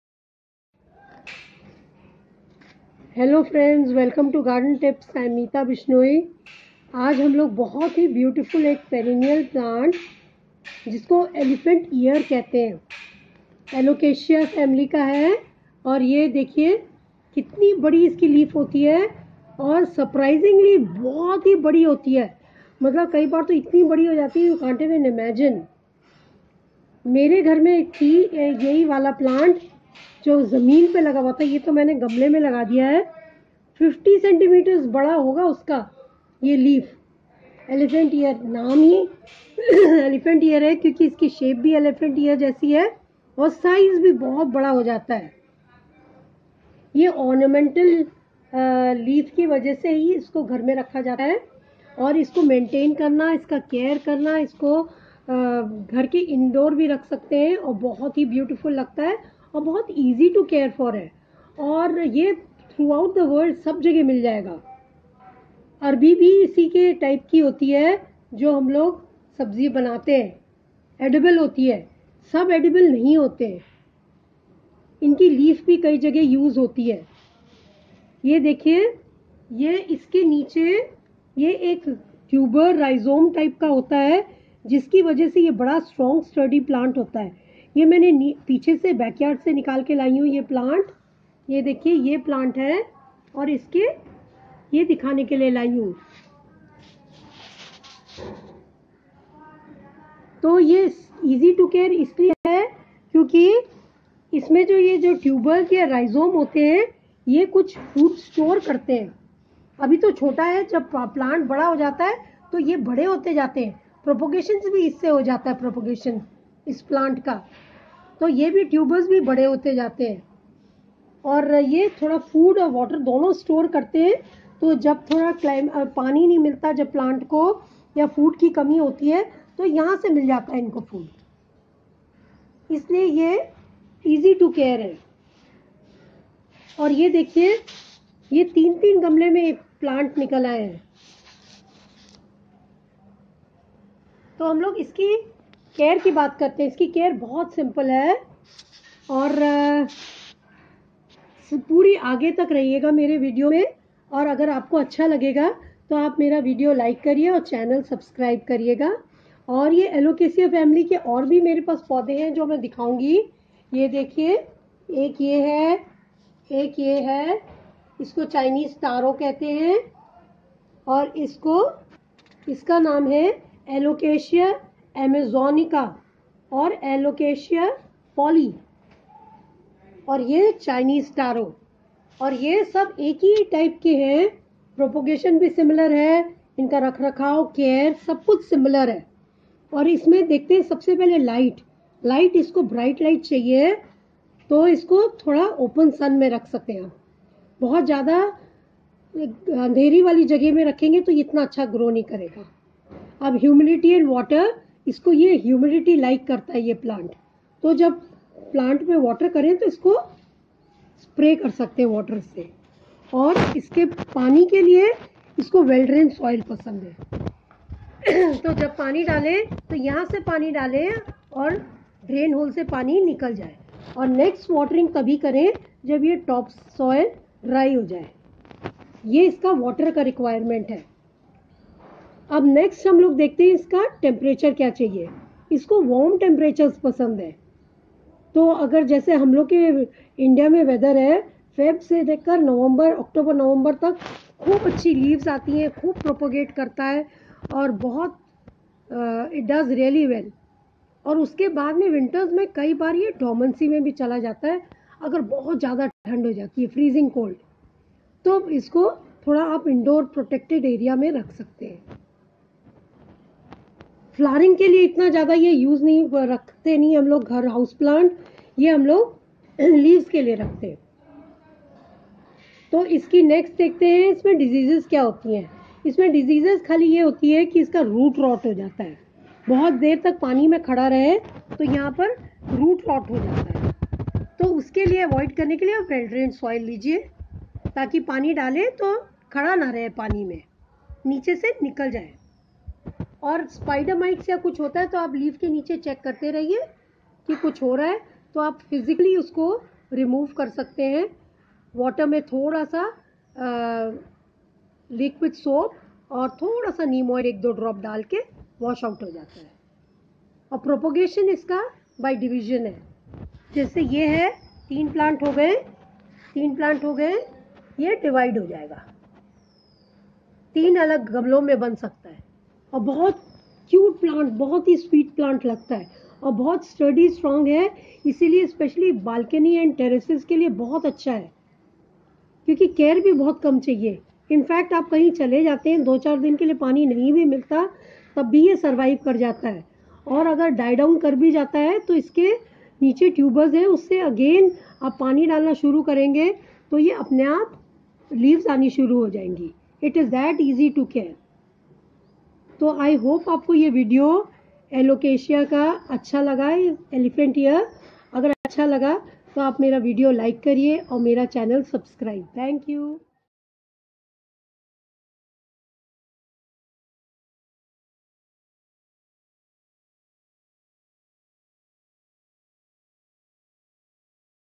How to care for Elephant ear plant//Alocasia plants
Elephant Ear plant is  an ornamental plant with beautiful leaves. Other plants of Alocasia family also shown in video. All of these are beautiful because of their foliage.Also included in video is how to take care of these plants